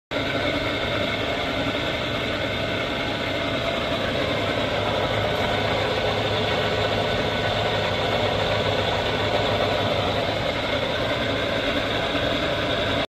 Used 12"x12" Side Entry Chicago Stainless Steel Rotary Airlock Valve
Demonstration video available.  Shipping info:  one skid, 44x44x24.  Weight = 487 lbs.